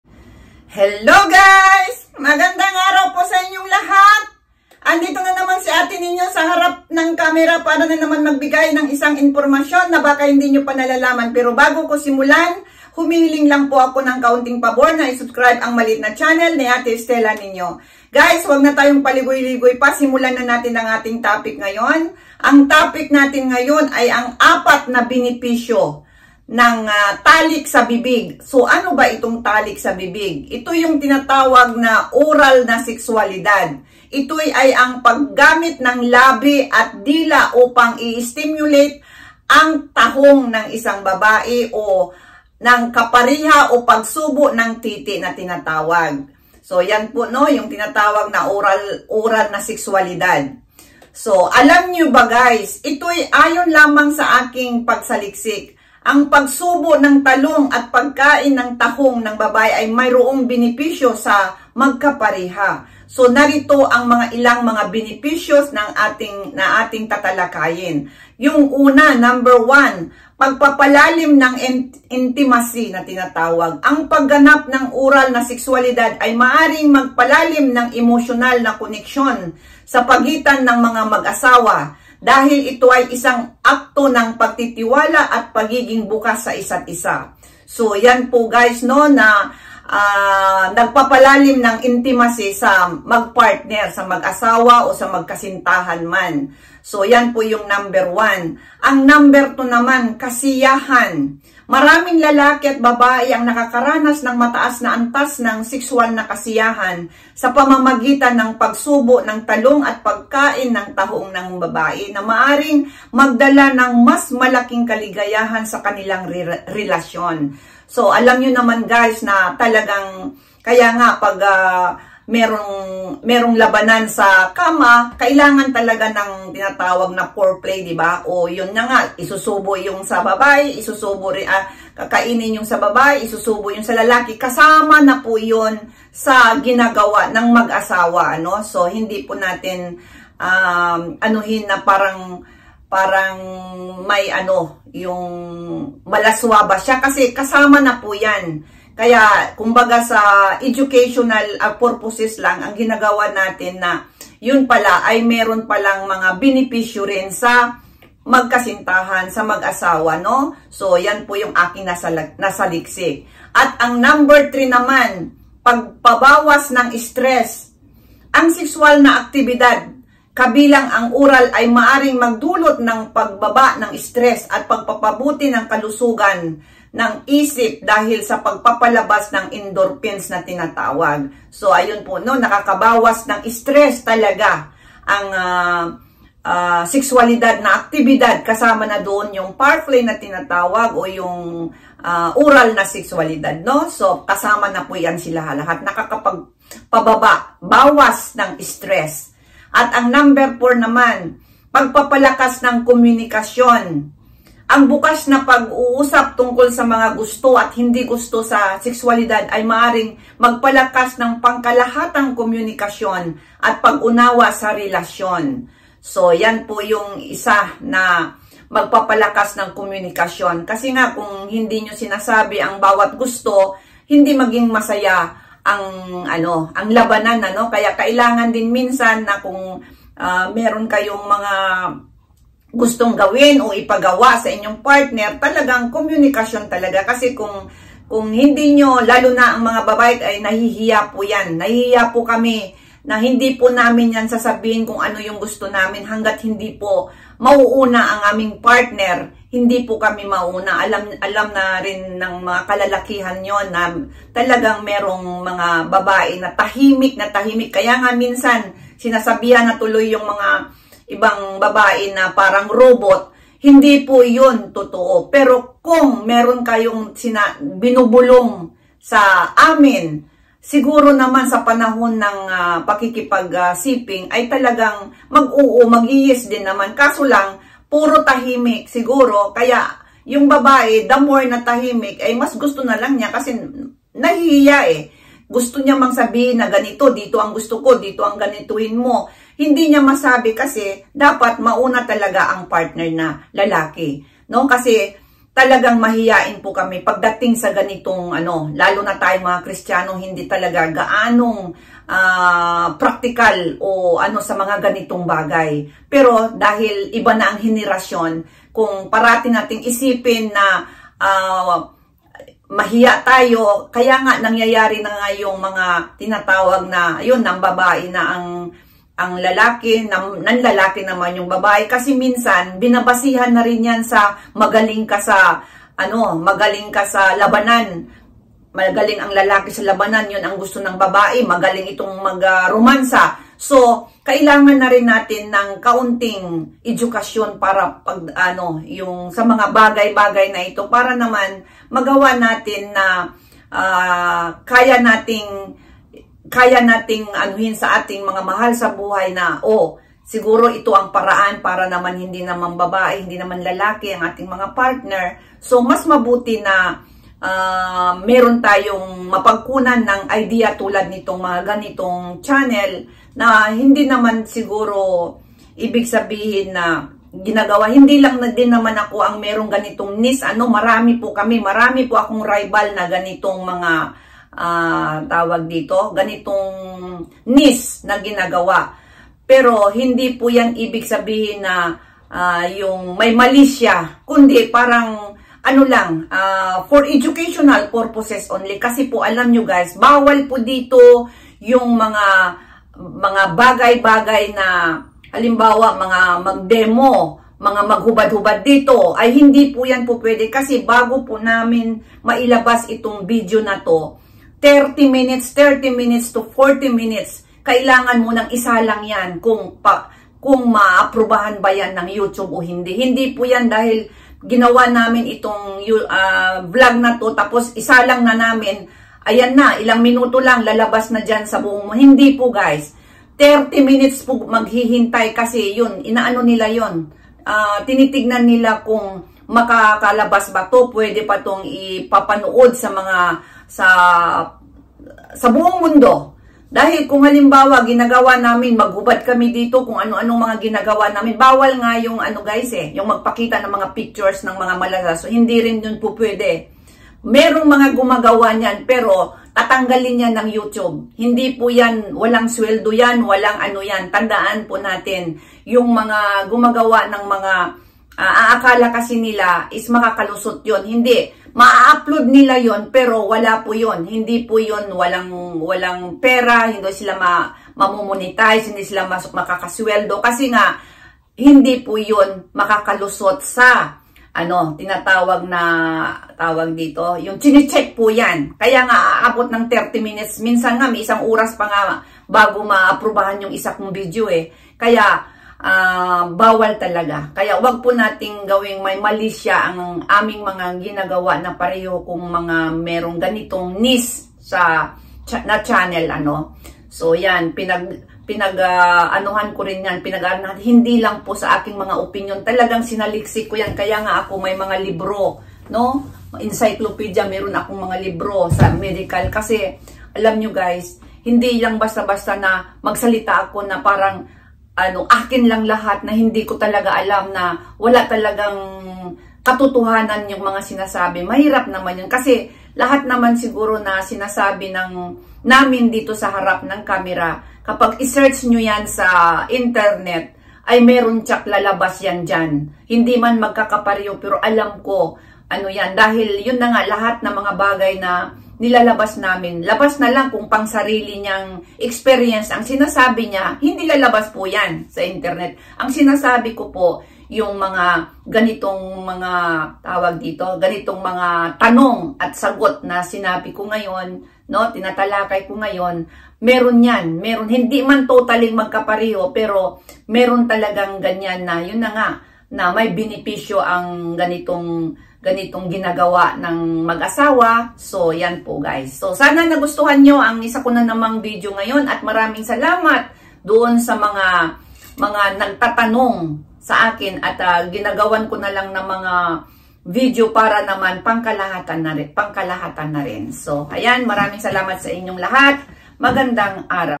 ORA'L S-X MAGUGULAT KA SA BENEPISYO KAPAG GINAWA ITO
May mga bagay na dapat gawin ng mag asawa para magkaroon ng matatag na relasyon ang pagsasama.Yan ang ating topic sa vlog natin for today.